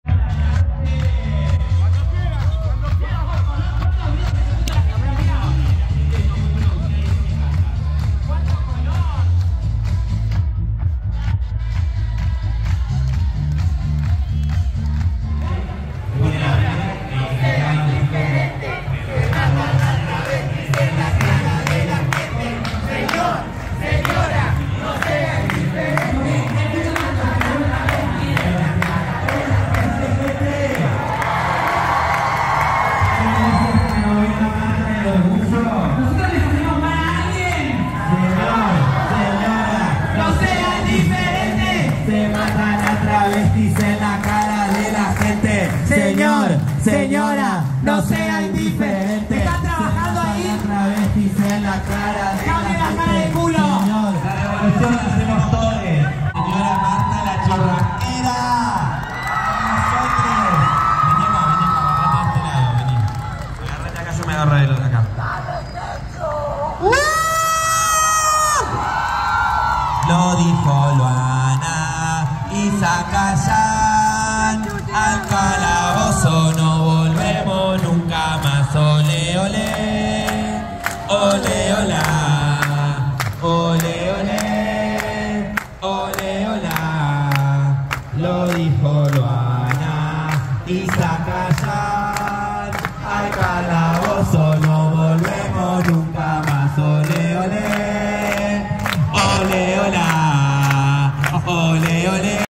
9na Marcha del orgullo TLGBINB de la provincia de Buenos Aires en La Plata.
Argentina 🇦🇷

9na Marcha del orgullo TLGBINB de la provincia de Buenos Aires en La Plata. Cobertura OTRANS. @claudiavasquezharo